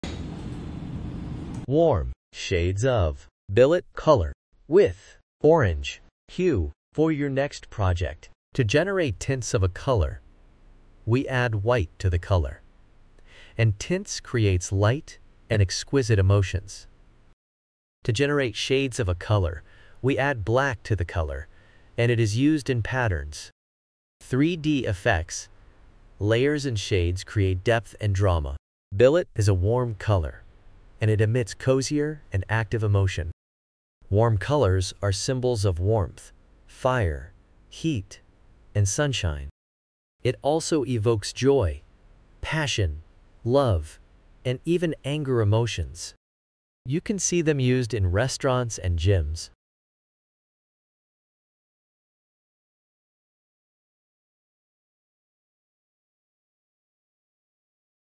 Shades & Tints of Billet color ad7c35 A Warm Orange color 996e2f #866029
Shades of Billet color ad7c35 Warm Shades of Billet color with Orange hue for your project!

Shades
Hex 996e2f

Tints
Hex b68a4b

Billet is a warm color, and it emits cozier and active emotion. Warm colors are symbols of warmth, fire, heat, and sunshine. It also evokes joy, passion, love, and even anger emotions. You can see them used in Restaurants and Gyms.